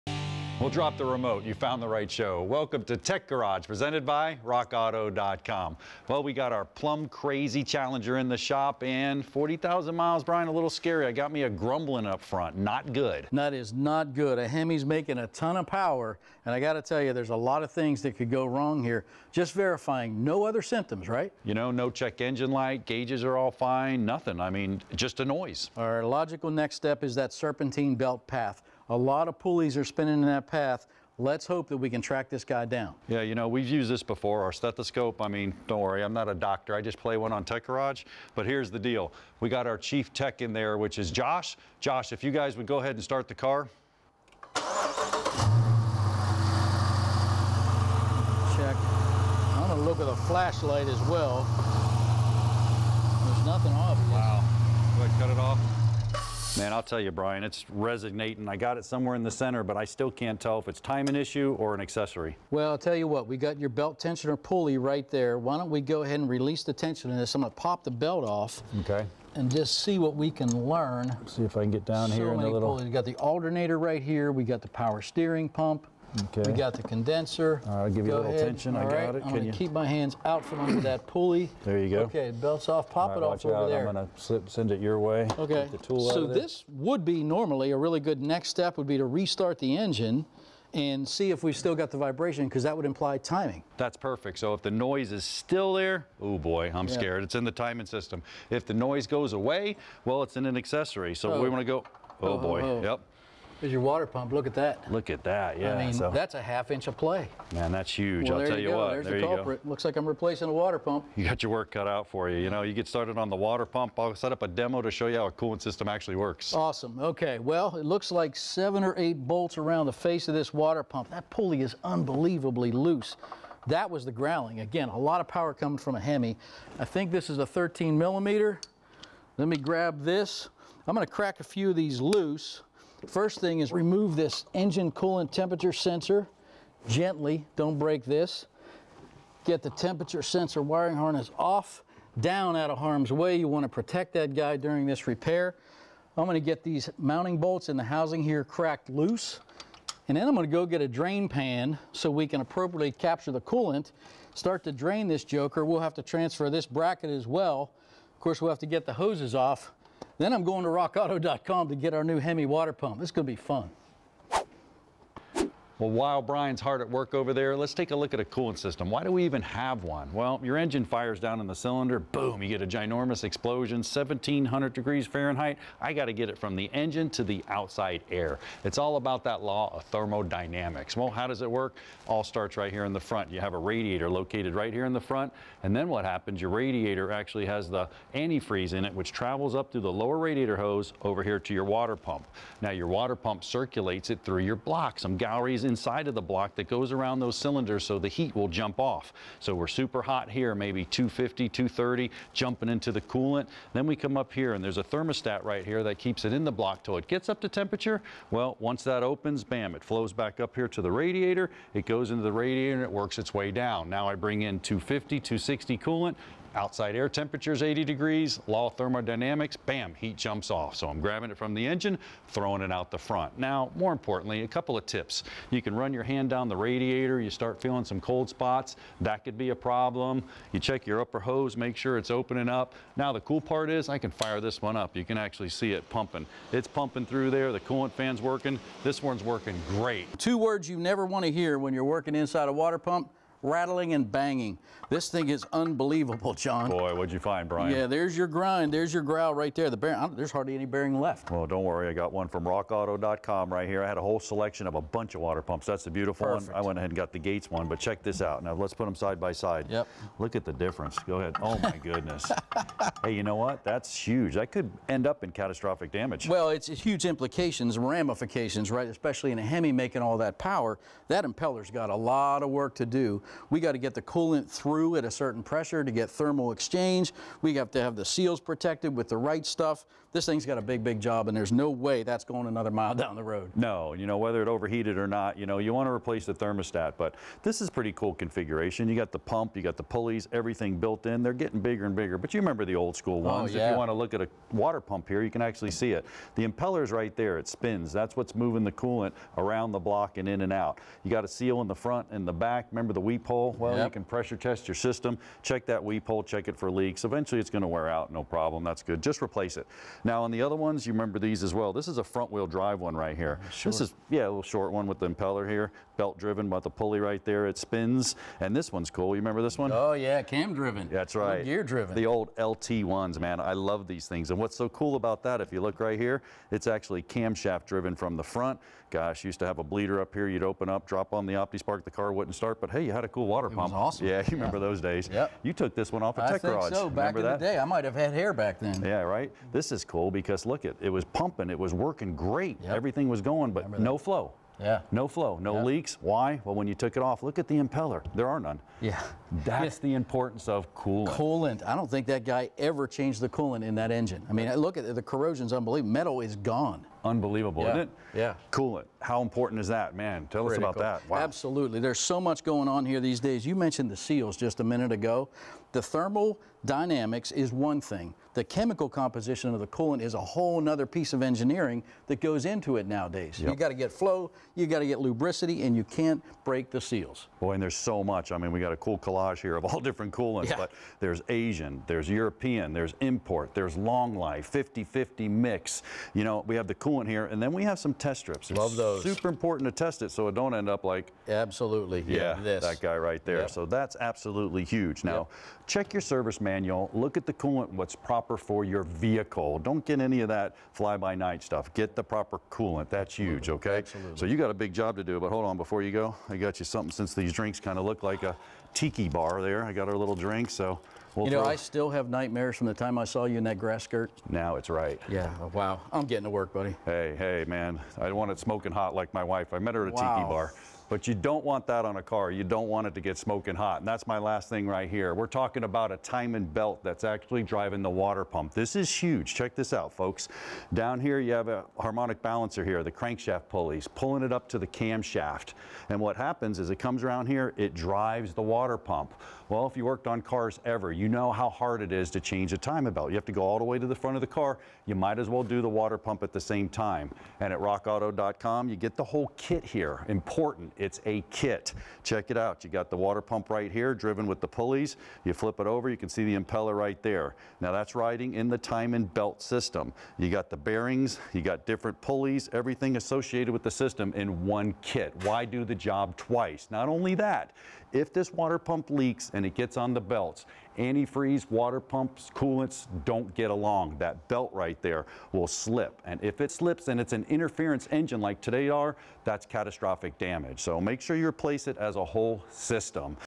S6 Water Pump Repair on Dodge Challenger & Cooling System Ops
How to diagnose (this one is obvious) a bad water pump and replace it on a 5.7 Hemi.   And a inside look at the cooling system via a demo we built